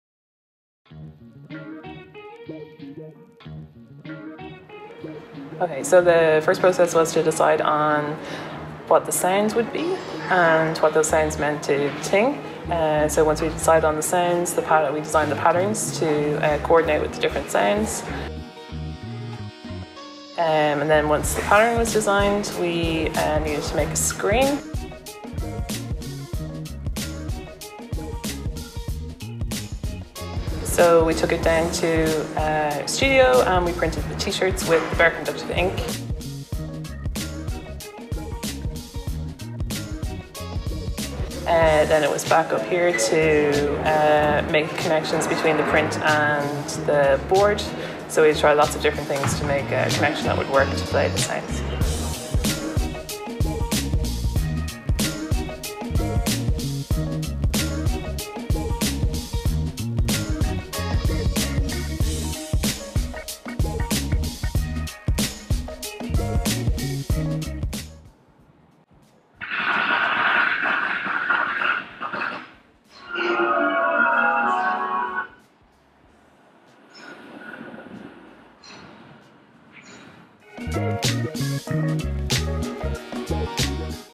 Bare Conductive - T-Shirt
Here is a quick process of the making of an interactive T-Shirt using the Bare Conductive ink.